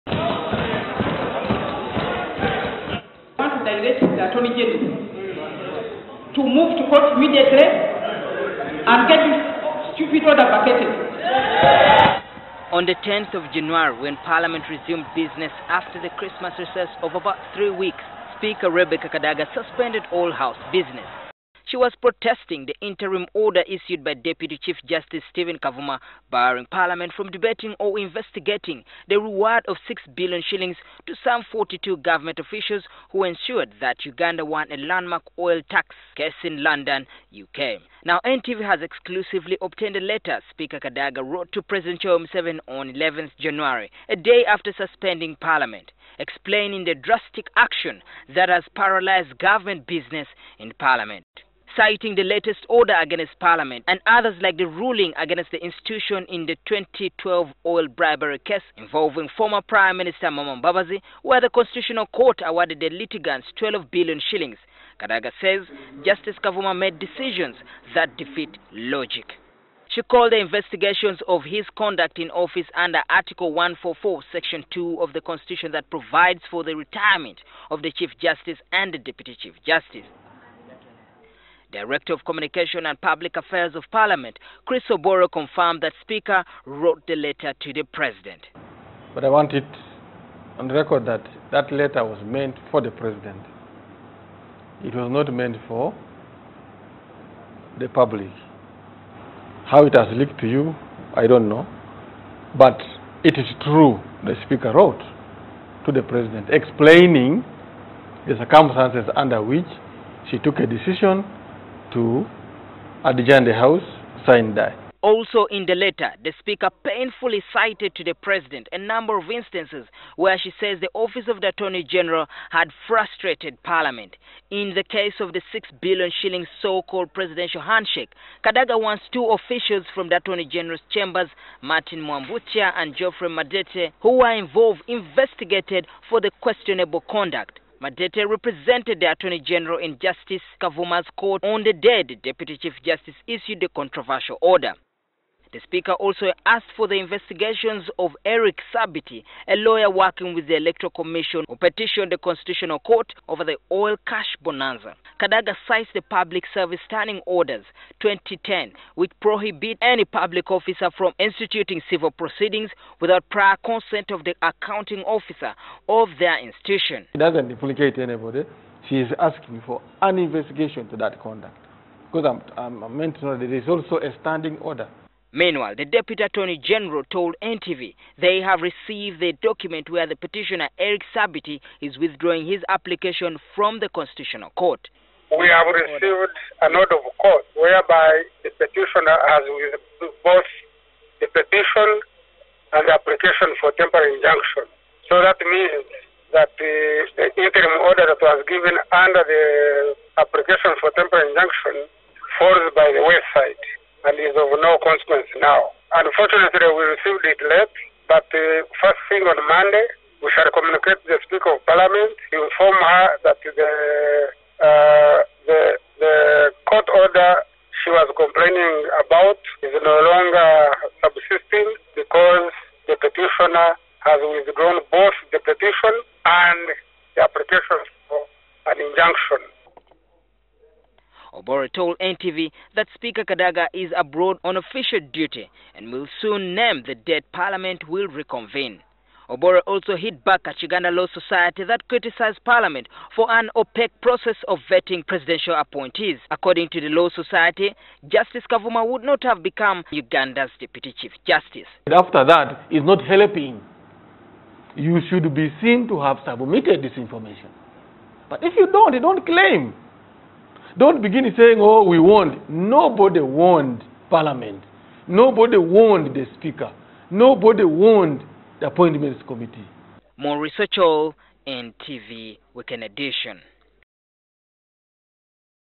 Kadaga writes to President criticising Justice Kavuma for interfering in Parliamentary business
Speaker of Parliament, Rebecca Kadaga, has spoken out strongly against Deputy Chief Justice, Stephen Kavuma, and called for an investigation of his conduct in office.

In a strongly worded letter to President Yoweri Museveni that NTV has obtained, Speaker Kadaga said Justice Kavuma has made decisions, even in the past  that defeat logic.
The Speaker wrote to the President explaining why she indefinitely suspended Parliamentary business following Justice Kavuma's interim order. Kadaga also called for an investigation into the conduct of three civil servants who were involved in the application filed before Justice Kavuma, who is also the head of the Constitutional Court.